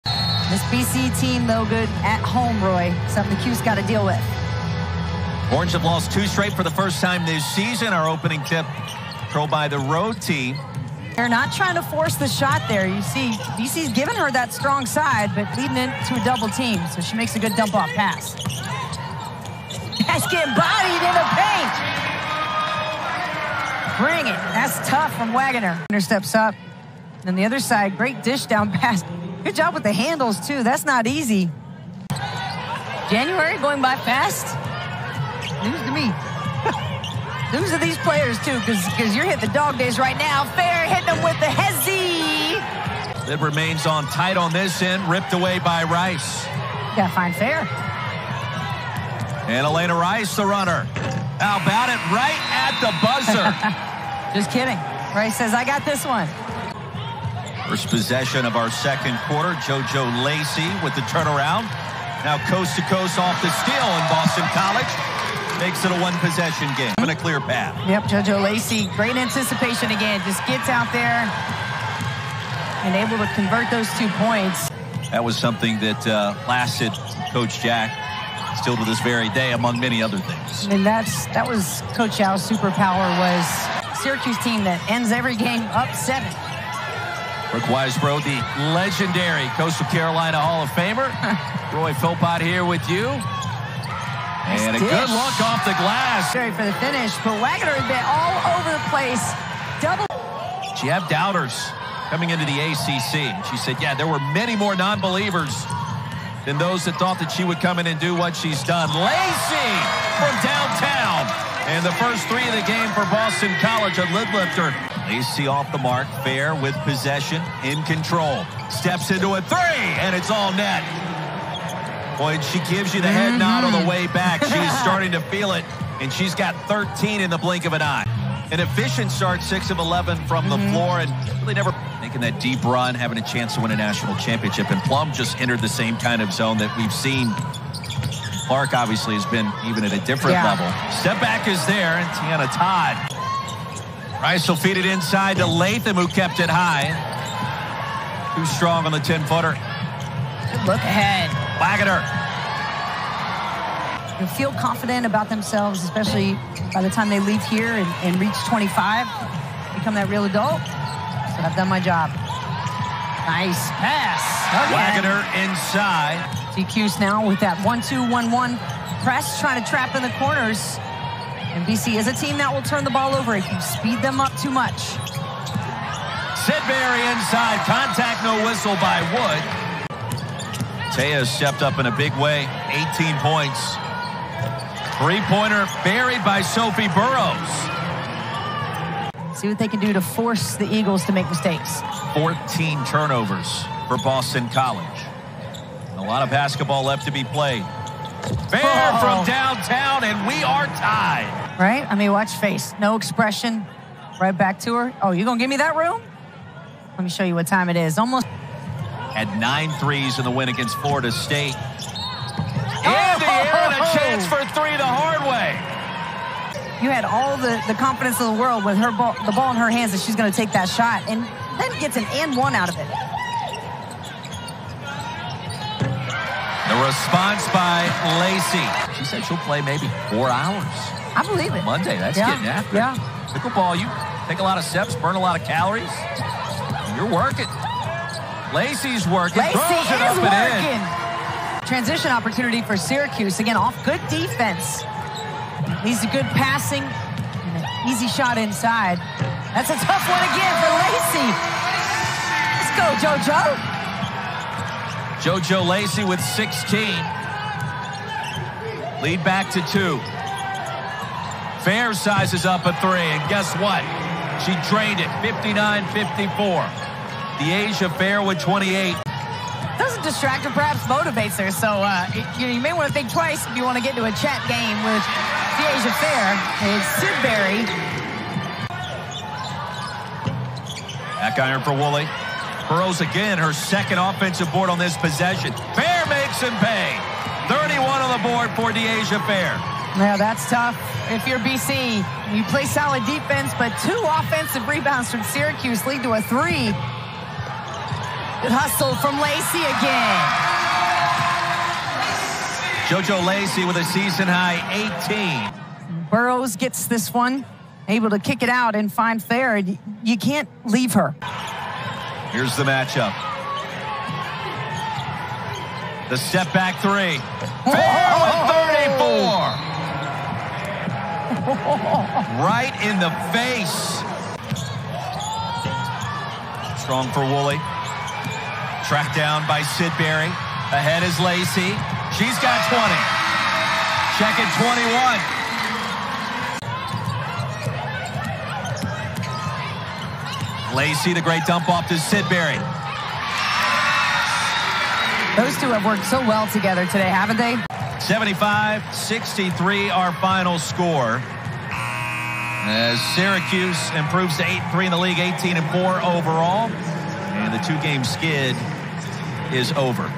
Syracuse vs. Boston College Game Highlights | 2023-24 ACC Women's Basketball
The #21 Orange came from behind to get the 75-63 win on the road against the Eagles. This game was dominated by a great performance from Dyaisha Fair, who scored over half of Syracuse's total points. Fair scored 38 total points in the game on 13-25 shooting, including 5-11 from three-point range. Alaina Rice added 10 points and four steals for the Orange. Teya Sidberry led Boston College with 23 points and 11 rebounds. JoJo Lacy scored 21 points off the bench for the Eagles.